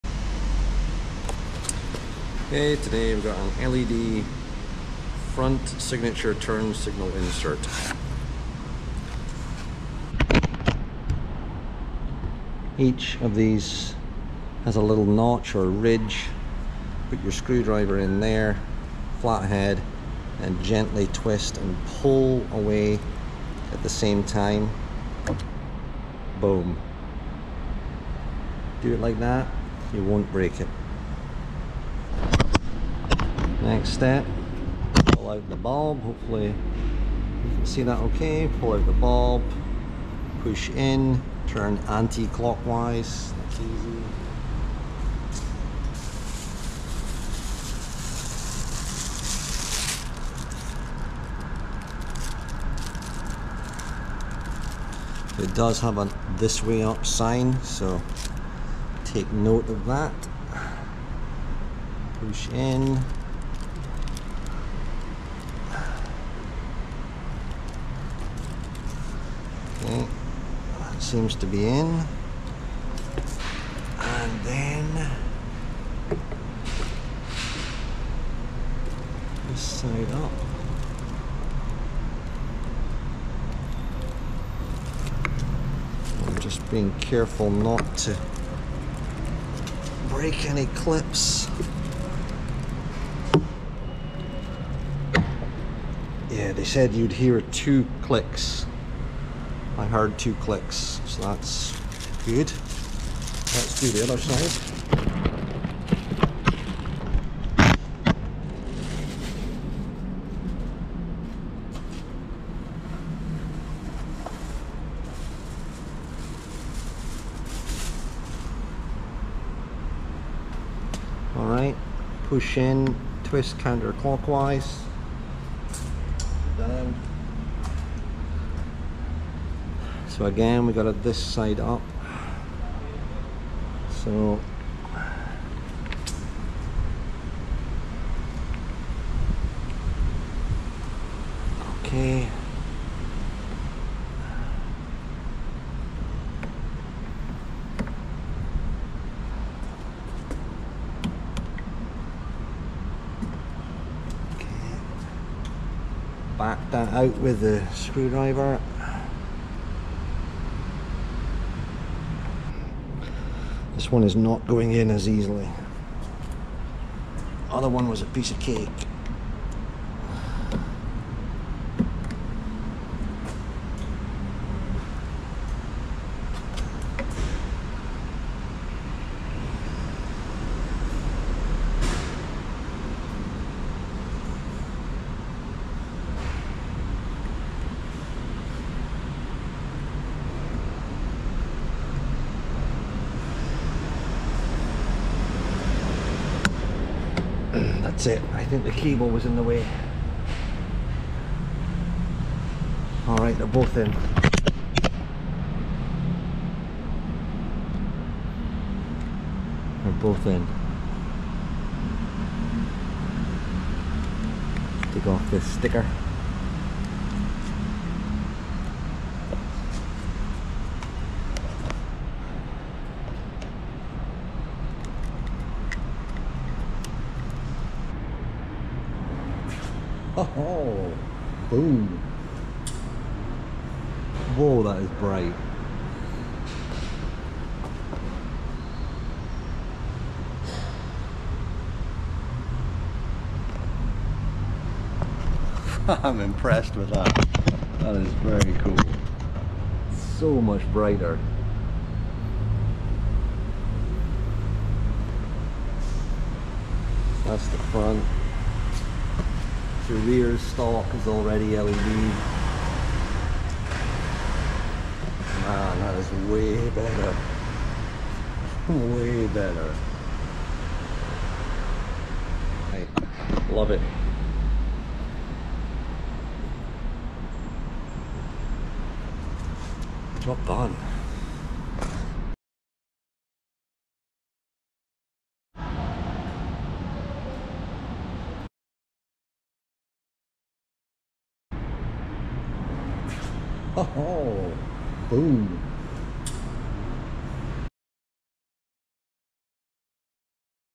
Harley Davidson Front LED install
Harley Davidson Front LED install on my Fatboy 114, super easy and much brighter.